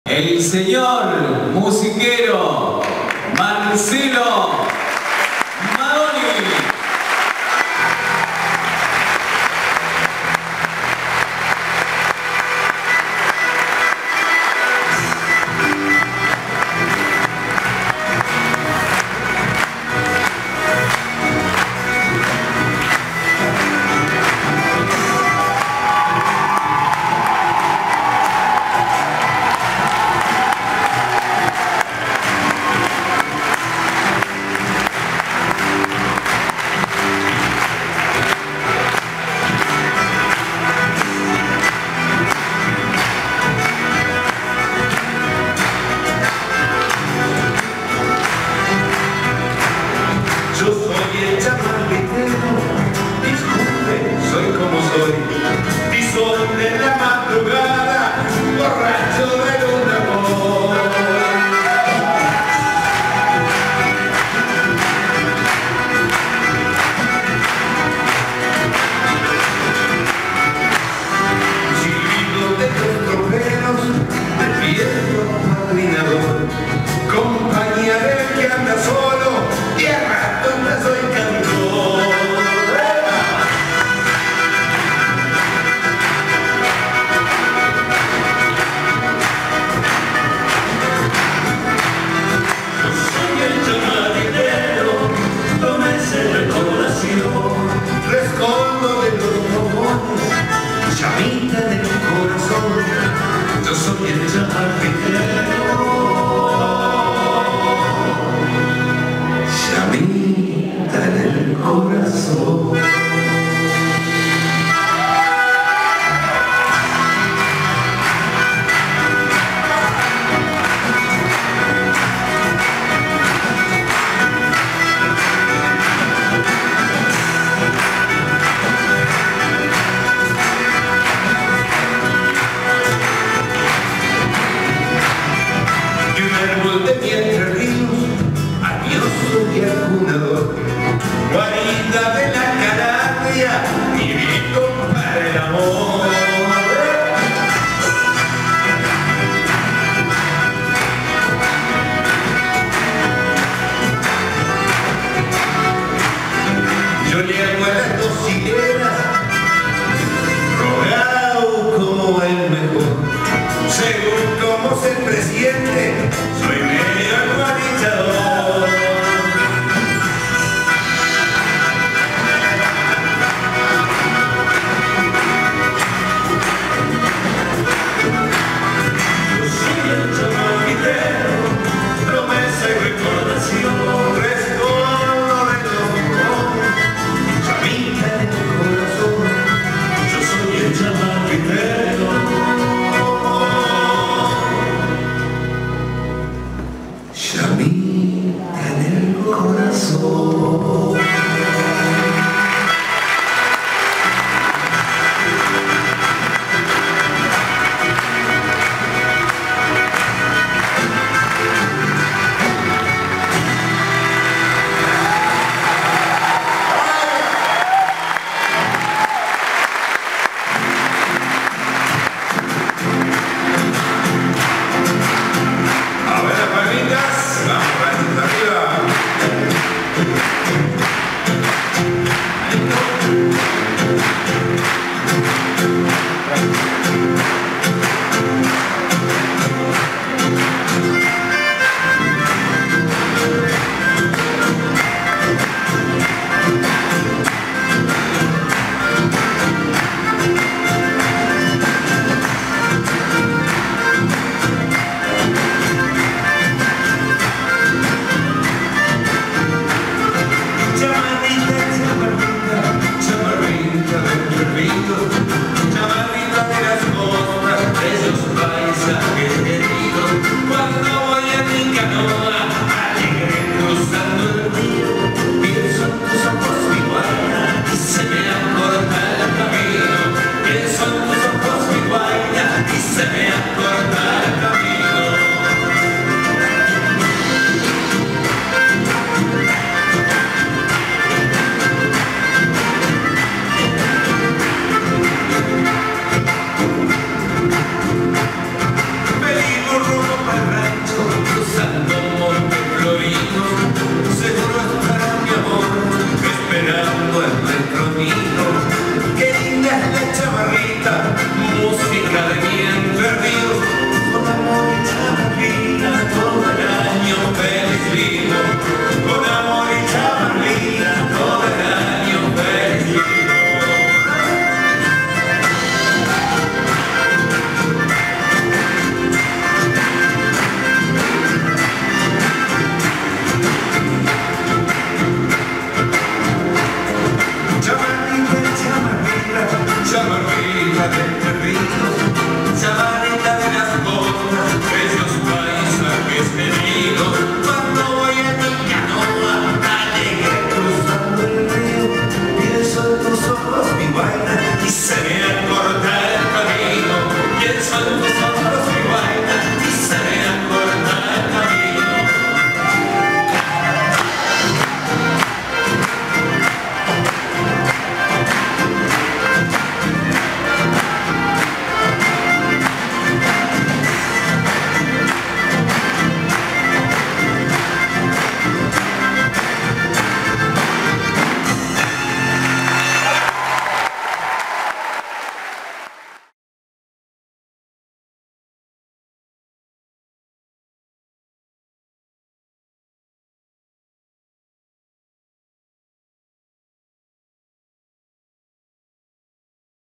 Marcelo Maddoni - Quiero Cantarte Entre Rios - Parte 1
Gracias a todos los que se hicieron presente este 7 de marzo en el Cine Teatro de la Sociedad Unión Italiana de Ranchos, para presenciar este espectáculo ¨Quiero Cantarte Entre Rios¨